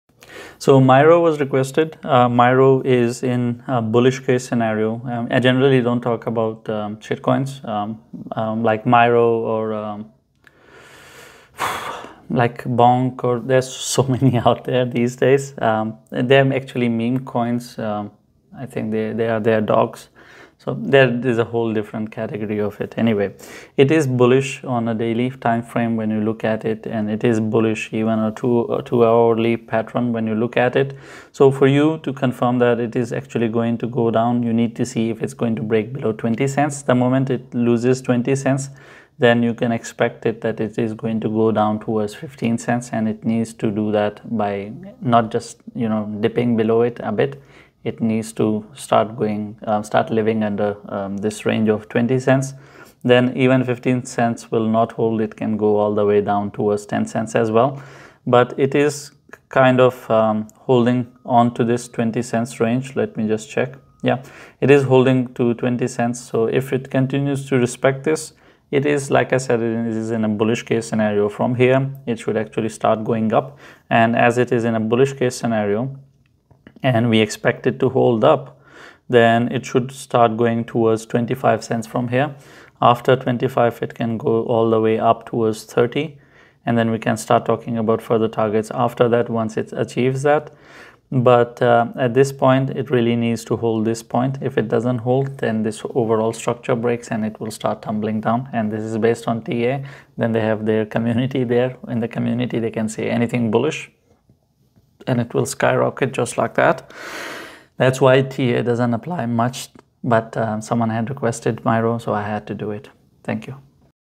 MYRO Coin Market Outlook as of 19 Jan 2024
MYROCoin

Get the latest insights into the MYRO coin market for January 19, 2024. In today's update, we delve into the current market dynamics, provide detailed technical analysis, and highlight key news affecting MYRO's value. Will MYRO continue its market trajectory, or is it facing a potential shift? Tune in for our expert analysis and predictions in the fast-paced world of digital currencies.

Your engagement and feedback are crucial for deepening our understanding of the cryptocurrency market's future.